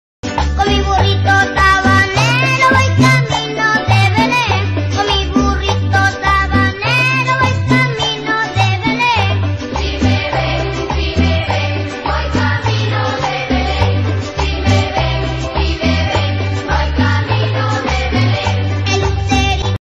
Shrek rides donkey
Shrek rides donkey to the moon